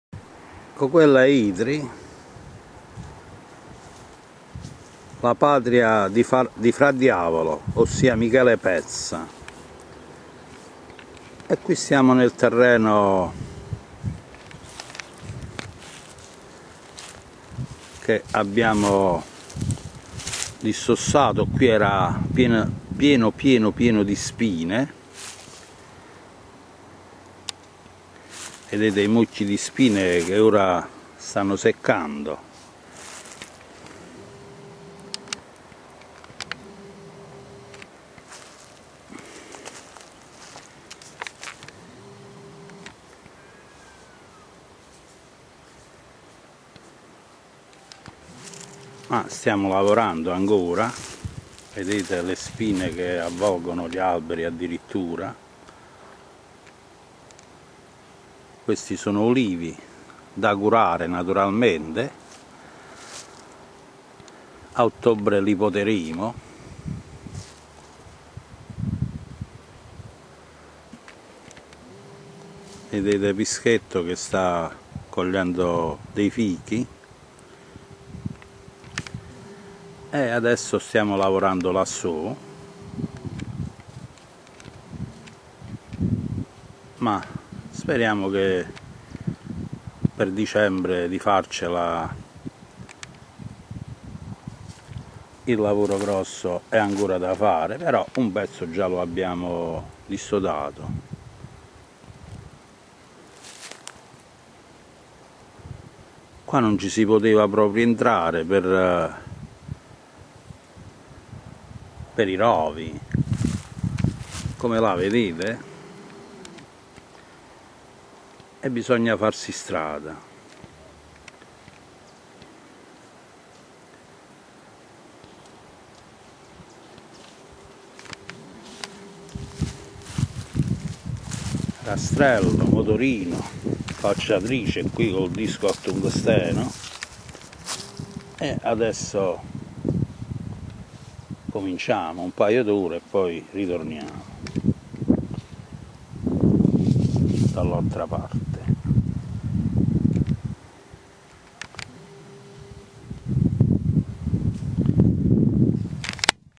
Gaeta.L'uliveto da dissodare dalle spine e dai rovi.
Per rinnovare la tradizione dei padri, stiamo lavorando in un uliveto abbandonato da 20 anni. Le spine sono alte due metri, i rovi abbondanti. La gente è disoccupata e si arrangia come può. Le olive di Gaeta, famose in tutto il mondo, potrebbero dare lustro alla economia della zona,come in passato.